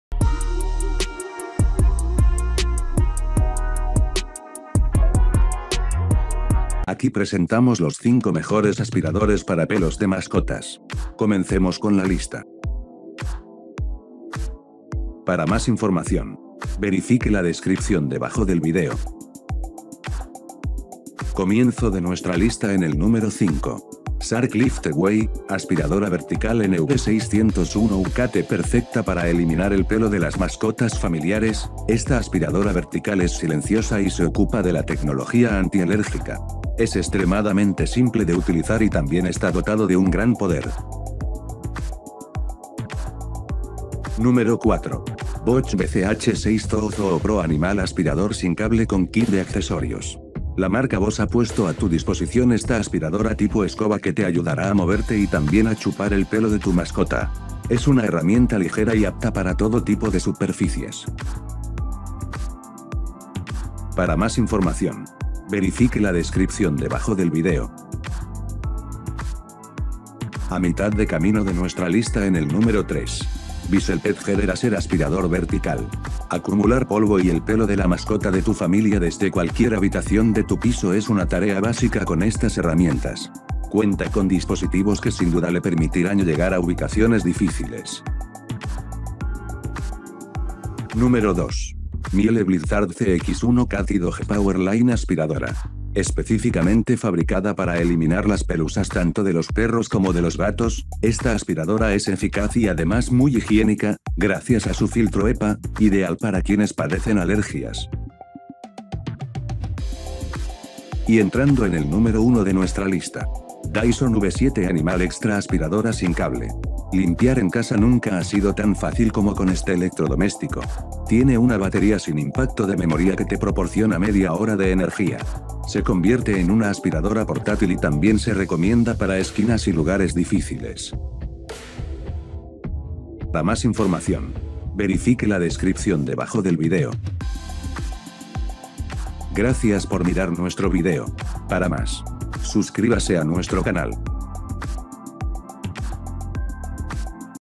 Los 5 Mejores Aspiradores para Pelos de Mascotas
Aquí está la lista de Los 5 Mejores Aspiradores para Pelos de Mascotas.

01. Dyson V7 Animal Extra Aspiradora sin Cable

02. Miele Blizzard CX1 Cat y Dog POWER Line Aspiradora

03. BISSELL Pet Hair Eraser Aspirador vertical

04. Bosch BCH6ZOOO Zooo ProAnimal Aspirador sin cable con kit de accesorios

05. Shark Lift-Away - Aspiradora Vertical [NV601UKT]